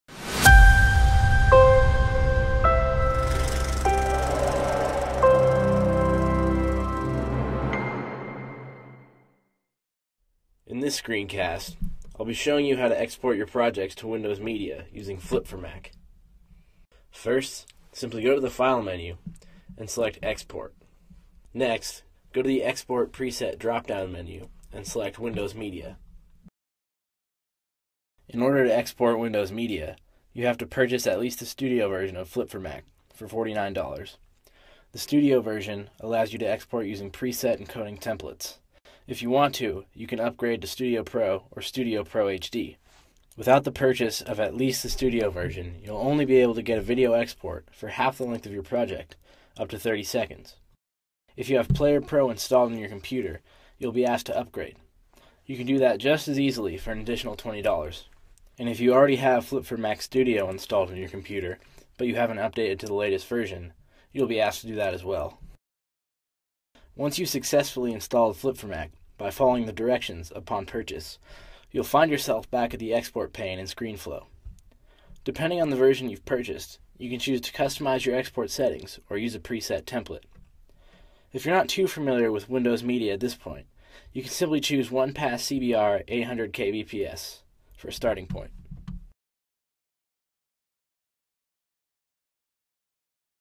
Exporting to Windows Media from ScreenFlow
This screencast will show you how to export your ScreenFlow projects to Windows Media using Flip4Mac.

Comments are not moderated by Telestream.  If you have a support question, go